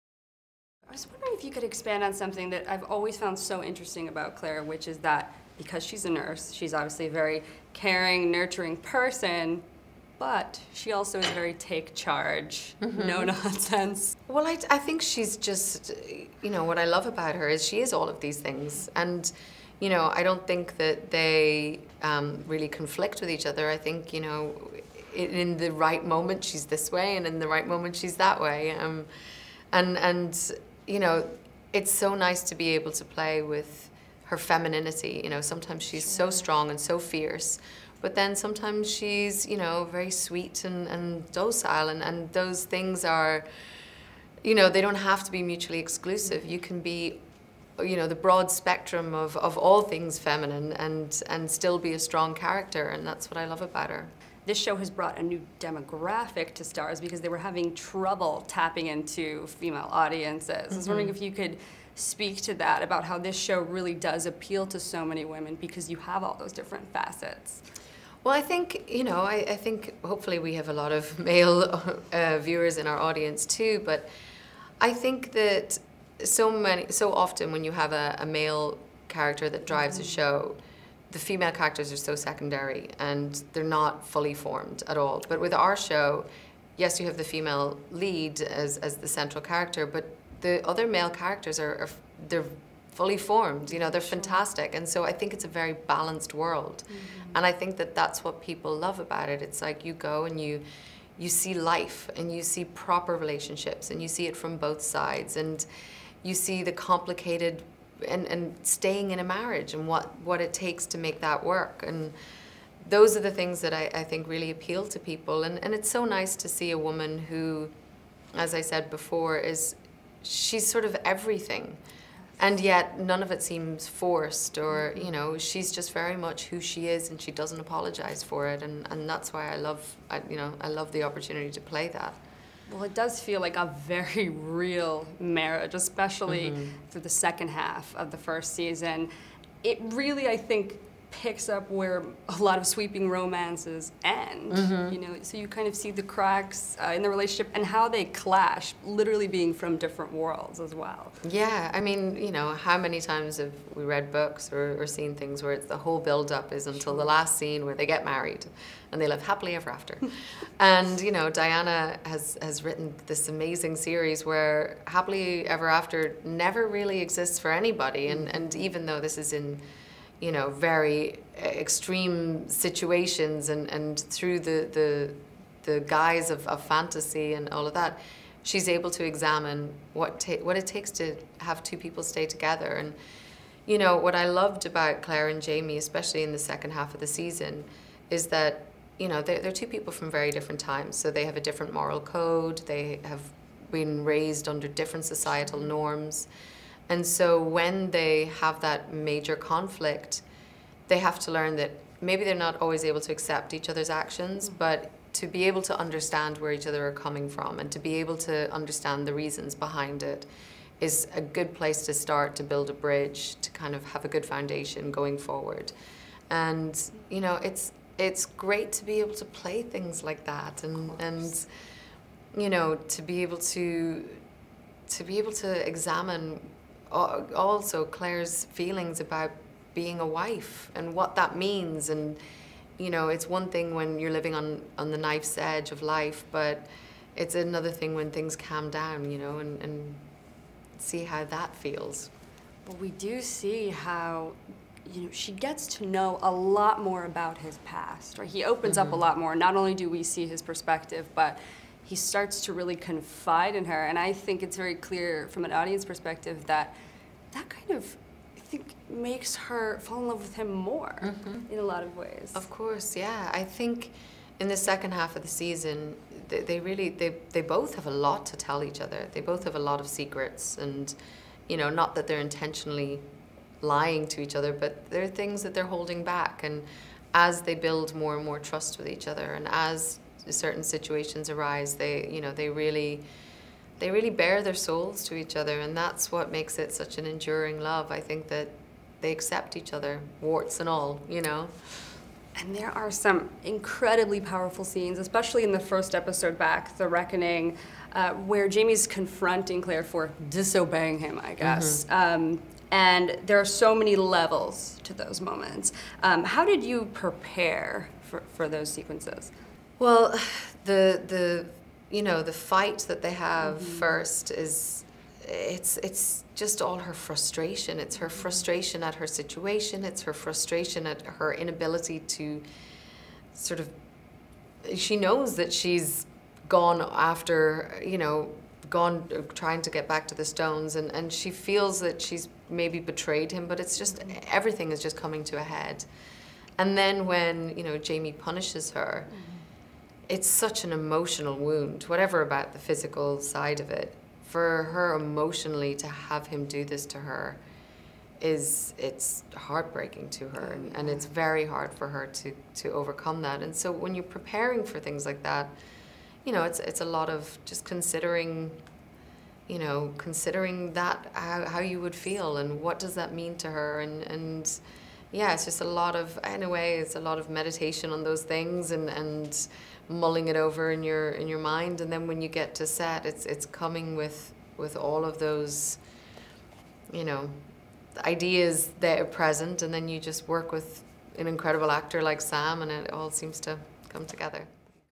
Outlander Interview: Caitriona Balfe On Claire and Jamie's Love Story and Strong Women on TV
Just in time for the midseason premiere of Outlander season one, Caitriona Balfe sits down to discuss Claire and Jamie's tumultuous marriage and how she's handled some of the difficult scenes the Starz hit has called for.